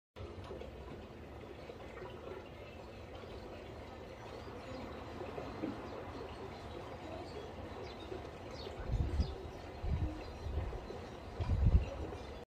Goldfish Pond - Life underwater
It is an amazing video of my goldfish living in the outdoor pond. Underwater video is captured while goldfish are actively hunting for food.